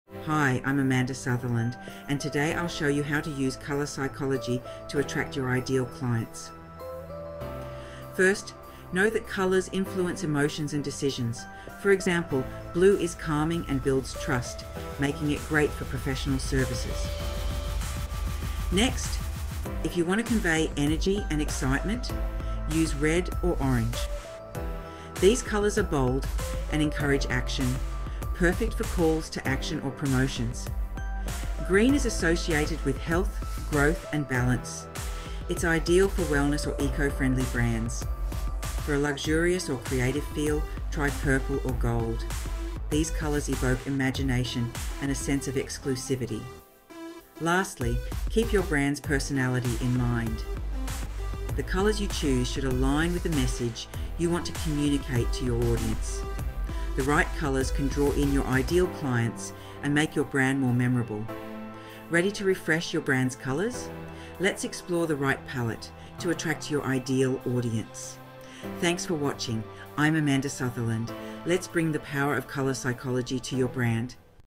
How to Use Colour Psychology to Attract Your Ideal Clients
Attract your ideal clients using colour psychology! Learn how blue, red, green, and other colours can influence emotions and drive engagement. Ready to refresh your brand’s palette? Let’s chat!